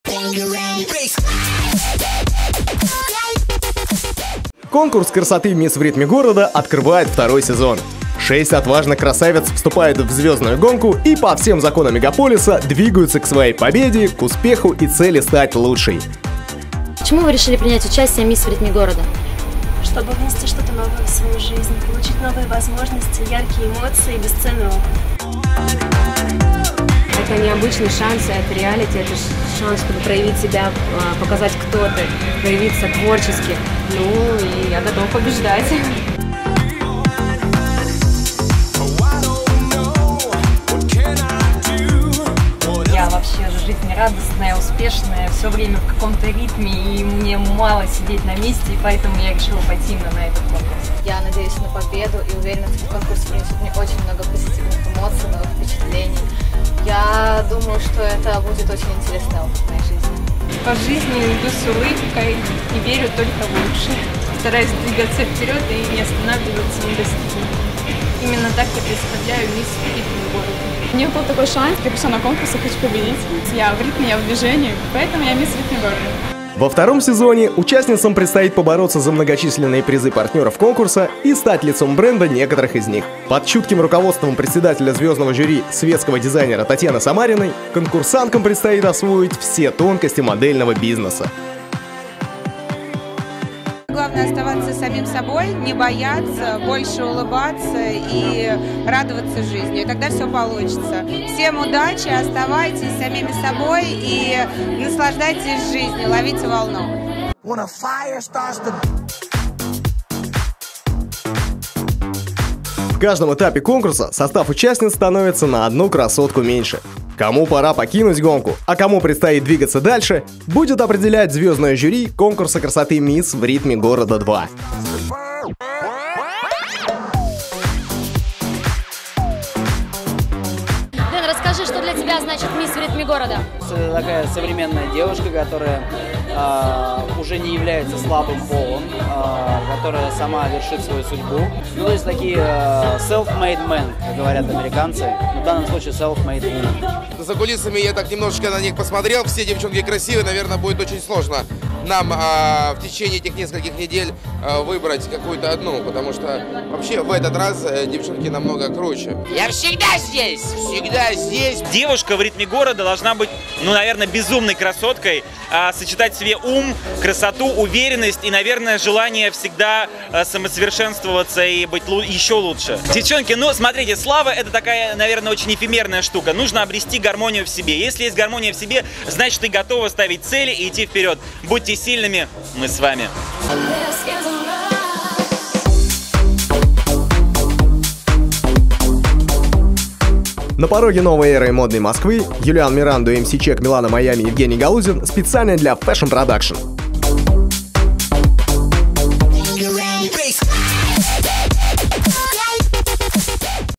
FPTV "Мисс в ритме города" 2 сезон
2-й сезон популярного Московского конкурса красоты "Мисс в ритме города". В жюри конкурса: Ведущий "Ю" и "Муз ТВ", Владиир Строжук, Певец, солист гр. "Челси", Денис Петров, Певец,пародист, Константин Кожевников, Певица, Ксюша Иванова, Продюсер, худ. рук. шоу "Diamond girls", Анна Исаева и др. Генеральный партнёр конкурса КРЦ "ЭРА". Информ поддержка: FPTV, FotoCoctail

fptv.pro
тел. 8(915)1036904